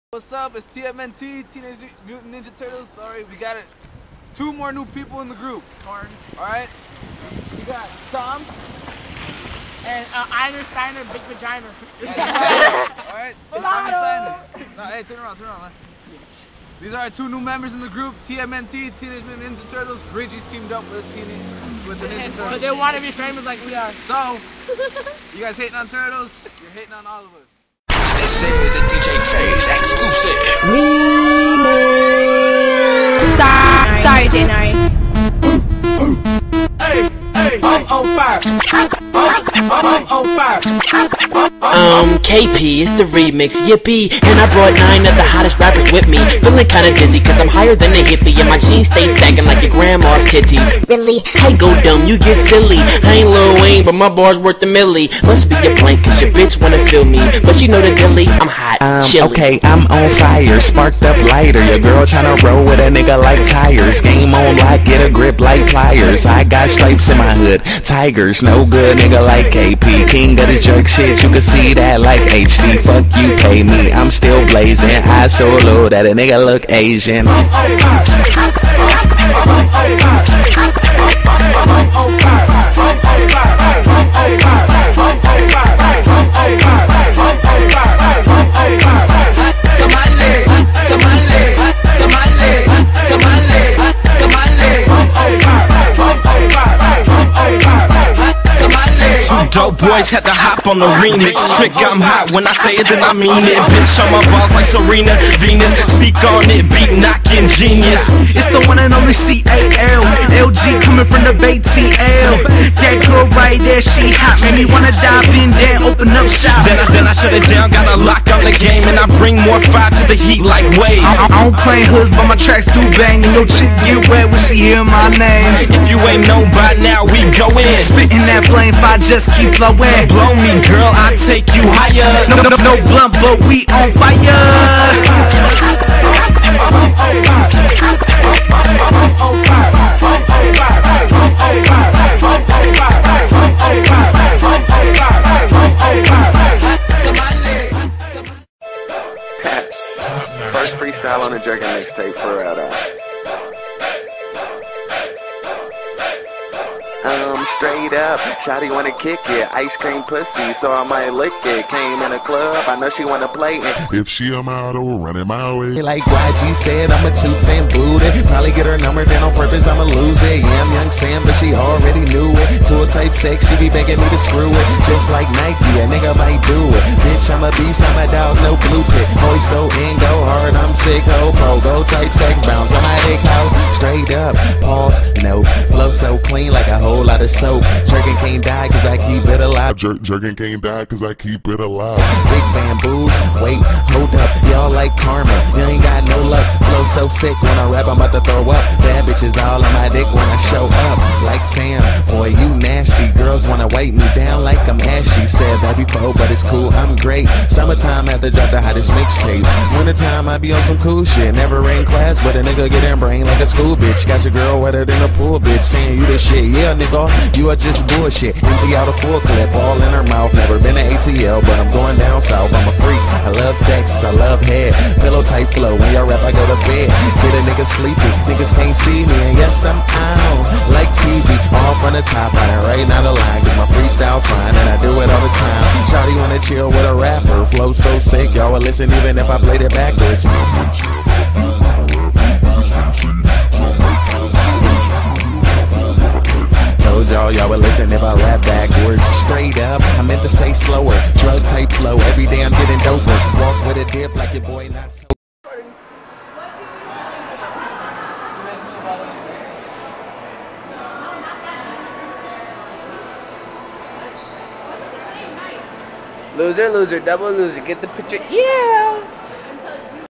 Ninja Turtles We Missed You Guys.. But Now We Back! (:
We Know We Know You guys missed us but now were back short little video to show you guys we never left... (: more videos coming soon!(;
- Ninja turtles
Shoutouts To
Tam And the Bro Sujnur!